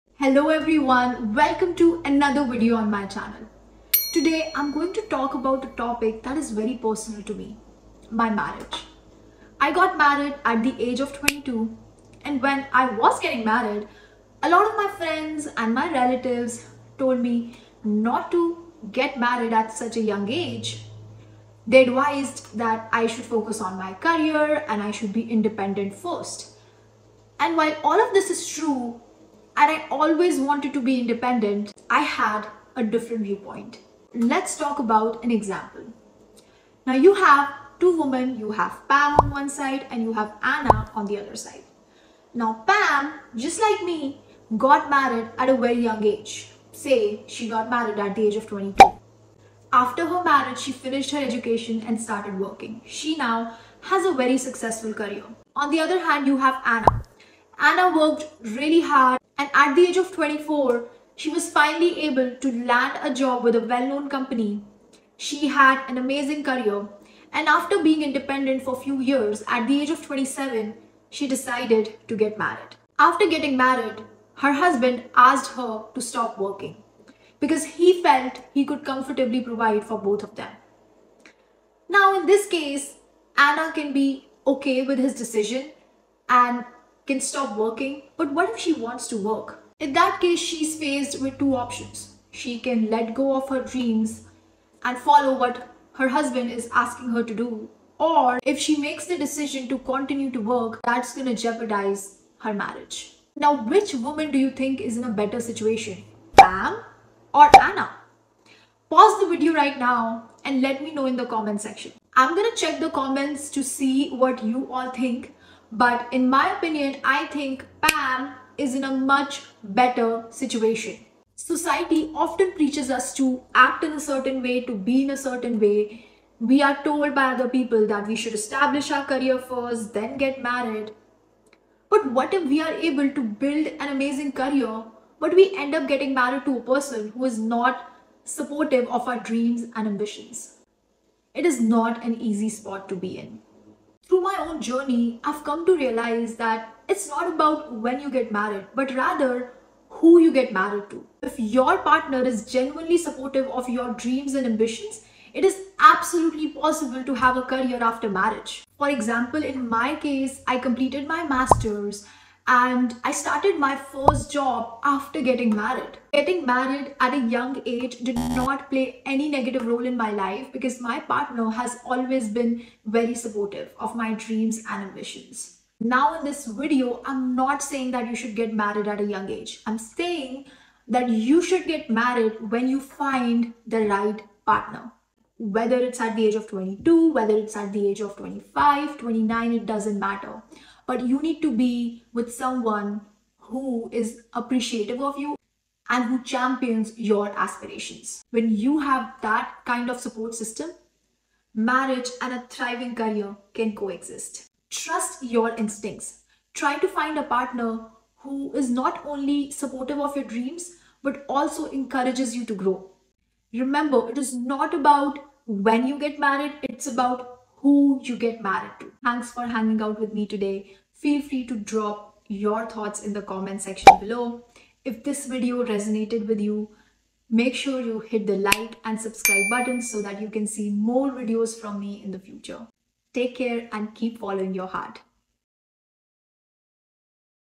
My experience getting married at the age of 22 | How has early marriage impacted my career?
Hello viewers,

In this video, I talk about how getting married at an early age impacted my career. I will answer questions such as did I make the right decision?, when is the right time to get married as per my experience?, how marriage impacted my career?